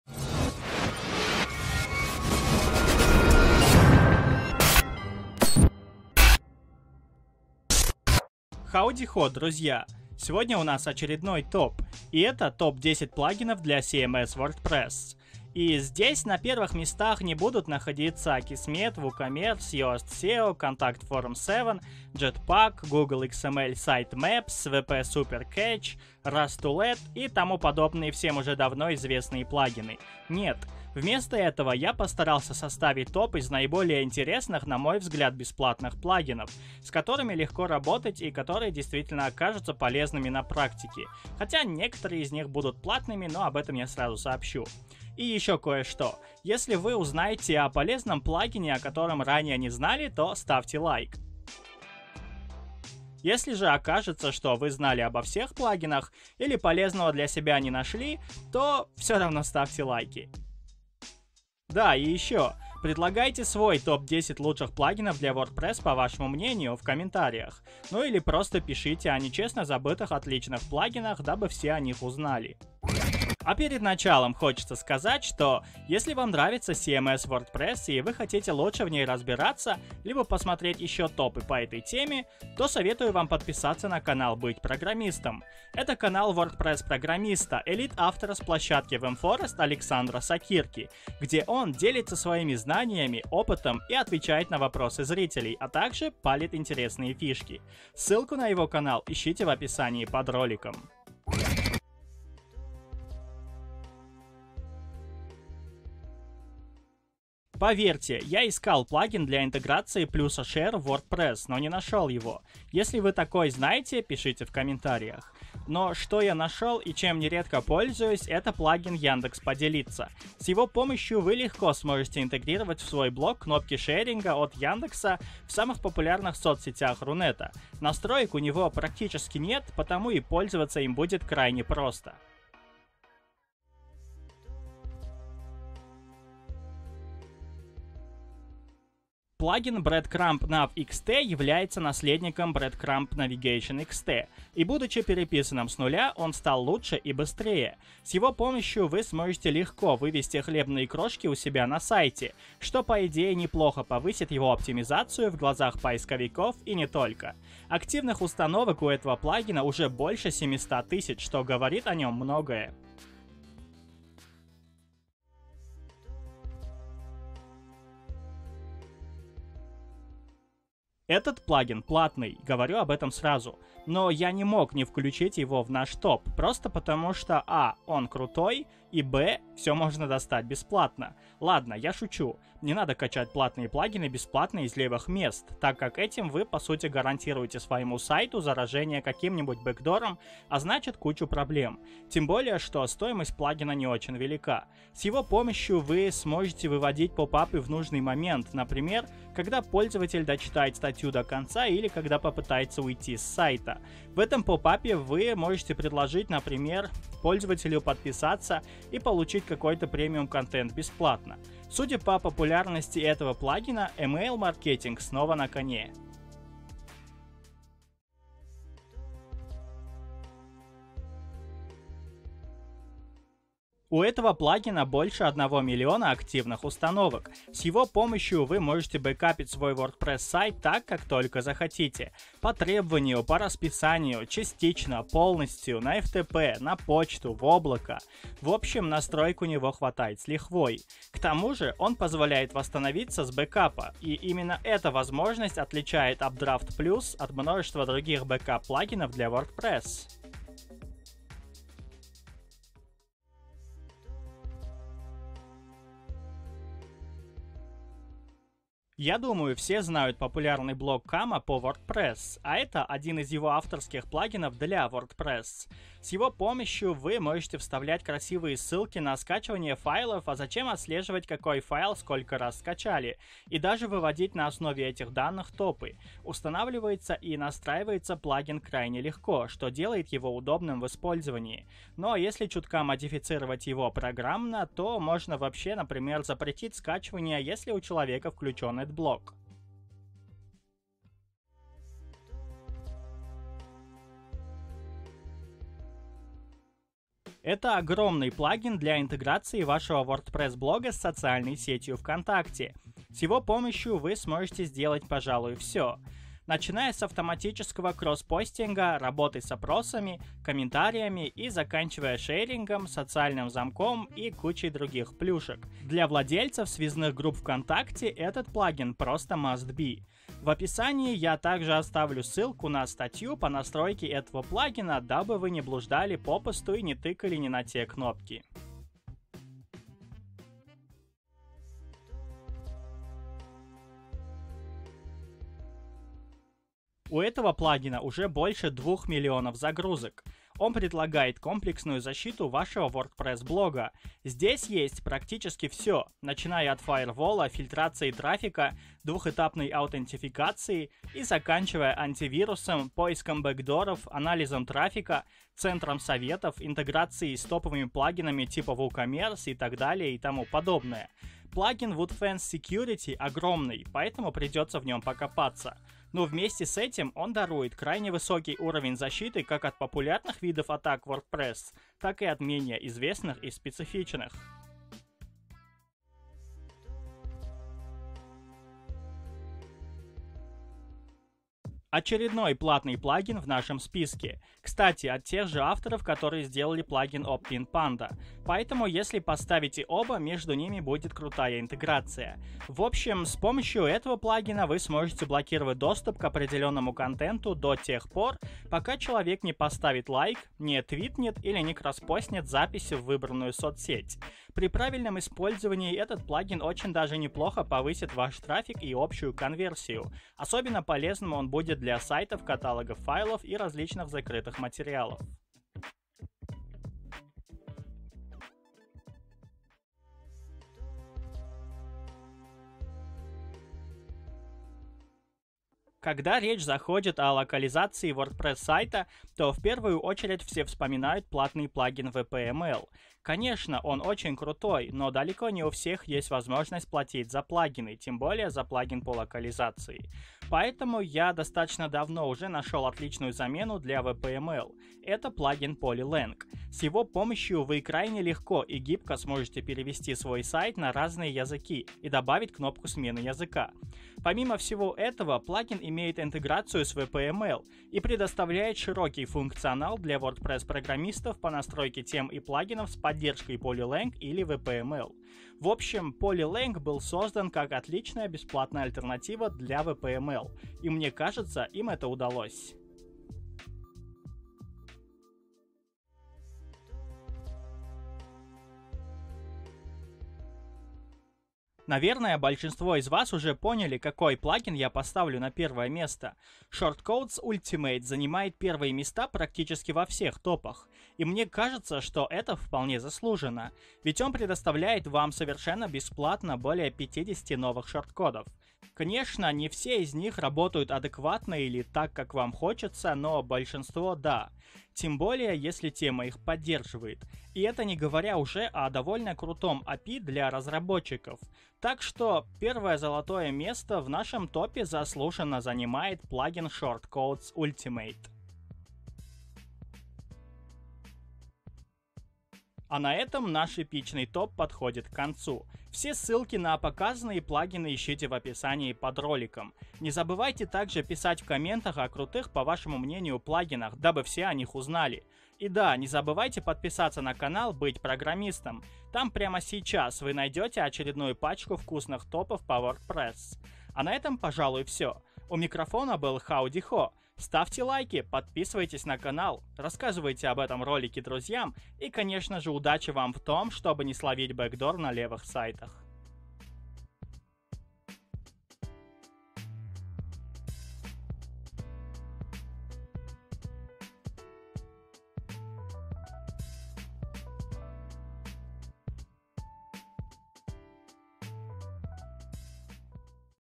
ТОП 10 WordPress плагинов
Топ 10 лучших WordPress плагинов для WordPress по мнению Хауди Хо.
Свои варианты предлагайте в комментариях.

😜 Подумываешь прикупить себе игру? ;)

+50 к удаче если ты:
✅ Подписался
✅ Оставил комментарий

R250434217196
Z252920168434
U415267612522